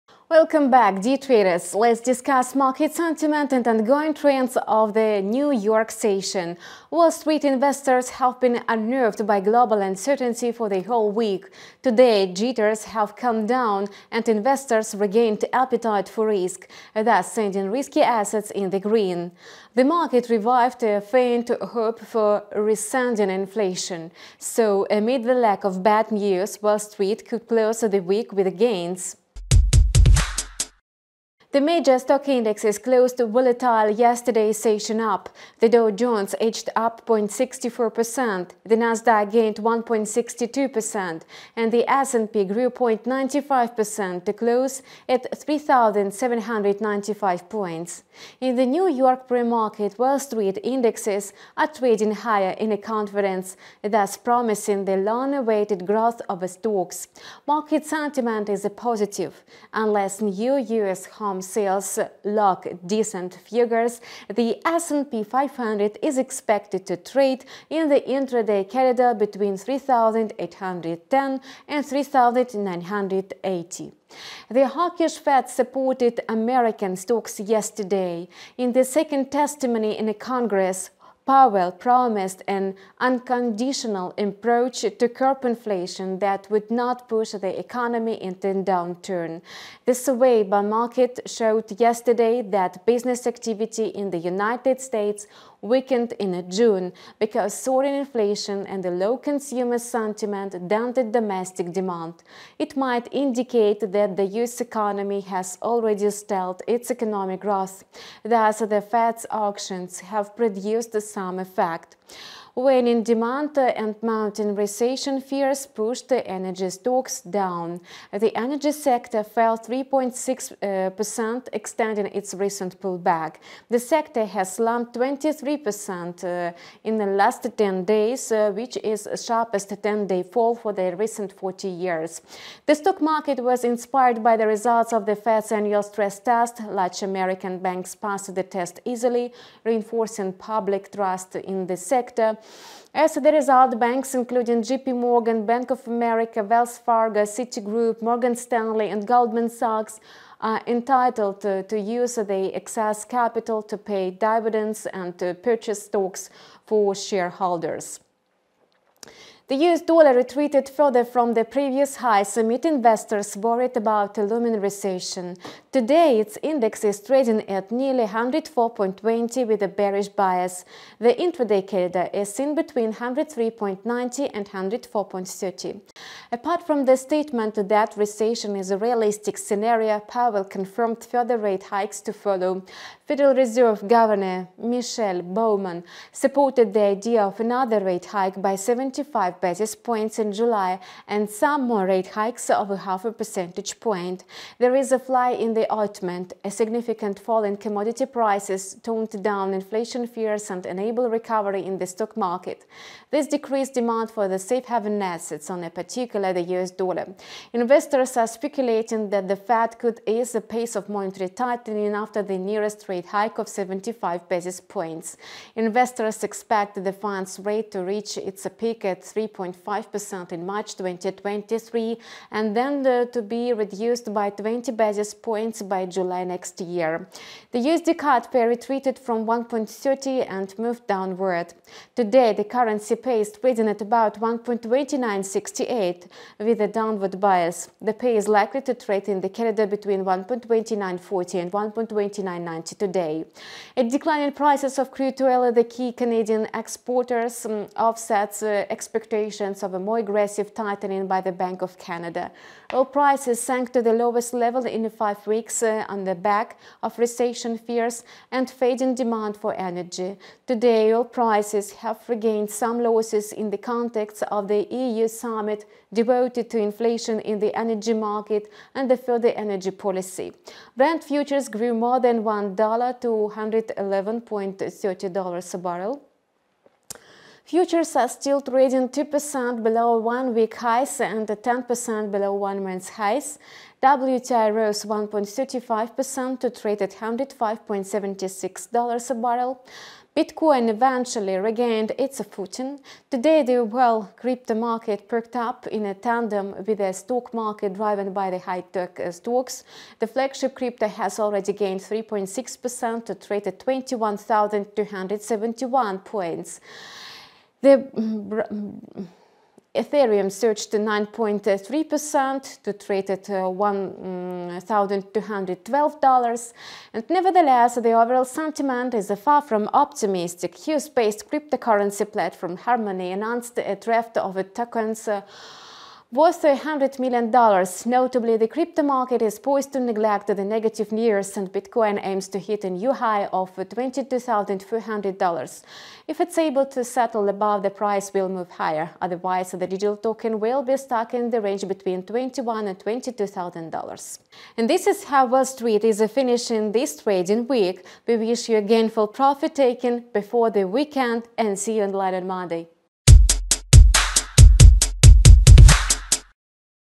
24.06.2022: Wall Street aims to close week with gains (S&P500, USD, CAD, Bitcoin)
Hi, dear traders! Let’s discuss market sentiment and ongoing trends of the New York session. Wall Street investors have been unnerved by global uncertainty for the whole week. Today jitters have calmed down and investors regained appetite for risk, thus sending risky assets in the green. The market revived a faint hope for receding inflation. So, amid the lack of bad news, Wall Street could close the week with gains
The major stock indices closed volatile yesterday’s session up. The Dow Jones edged up 0.64%. The Nasdaq gained 1.62%. The S&P 500 grew 0.95% to close at 3,795. In the New York pre-market, Wall Street indices are trading higher in confidence, thus promising the long-awaited growth of stocks. Market sentiment is positive. Unless new US home sales log decent figures, the S&P 500 is expected to trade in the intraday corridor between 3,810 and 3,980.

The hawkish Fed supported American stocks yesterday. In the second testimony in Congress, Powell promised an unconditional approach to curb inflation that would not push the economy into a downturn.
The survey by Markit showed yesterday that business activity in the US weakened in June because soaring inflation and low consumer sentiment dented domestic demand. It might indicate that the US economy has already stalled its economic growth. Thus, the Fed’s actions have produced some effect.
Waning demand and mounting recession fears pushed energy stocks down. The energy sector fell 3.8%, extending its recent pullback. The sector has slumped 23% in the last 10 days which is the sharpest 10-day fall for the recent 40 years.
The stock market was inspired by the results of the Fed’s annual stress test. Large American banks passed the test easily, reinforcing public trust in the sector. As a result, banks, including JPMorgan, Bank of America, Wells Fargo, Citigroup, Morgan Stanley, and Goldman Sachs are entitled to use their excess capital to pay dividends and purchase stocks for shareholders.
The US dollar retreated further from the previous highs amid investors’ worries about a looming recession. Today its index is trading at nearly 104.2 with a bearish bias. The intraday corridor is seen between 103.9 and 104.3.
Apart from the statement that recession is a realistic scenario, Powell confirmed further rate hikes to follow. Federal Reserve Governor Michelle Bowman supported the idea of another rate hike by 75 basis points in July and some more rate hikes of half a percentage point.
There is a fly in the ointment. A significant fall in commodity prices toned down inflation fears and enabled a recovery in the stock market. This decreased demand for safe haven assets, on particular the US dollar. Investors are speculating that the Fed could ease a pace of monetary tightening after the nearest rate hike of 75 basis points. Investors expect the funds rate to reach its peak at 3.5% in March 2023 and then to be reduced by 20 basis points by July next year.
The USD/CAD pair retreated from 1.3000 and moved downward. Today the currency pair is trading at about 1.2968 with the downward bias. The pair is likely to trade in the corridor between 1.2940 and 1.2990 today.
A decline in prices of crude oil, the key Canadian exports, offsets expectations of more aggressive tightening by the Bank of Canada. Oil prices sank to the lowest level in 5 weeks on the back of recession fears and fading demand for energy. Today, oil prices have regained some losses in the context of the EU summit devoted to inflation in the energy market and further energy policy. Brent futures grew more than 1 dollar to 111.30 dollars a barrel. Futures are still trading 2% below one-week highs and 10% below one-month highs. WTI rose 1.35% to trade at 105.76 dollars a barrel.
Bitcoin eventually regained its footing. Today the whole crypto market perked up in tandem with the stock market driven by high-tech stocks. The flagship crypto has already gained 3.6% to trade at 21,271.

00:00 Introduction
00:31 S&P500
01:14 Powell and the US stock market
01:52 Fed annual stress test results
02:38 USDX
02:55 The impact of the macroeconomic situation on the dollar
03:47 USD/USD
04:03 The impact of the macroeconomic situation on the Canadian dollar
04:36 BTC/USD
05:44 Results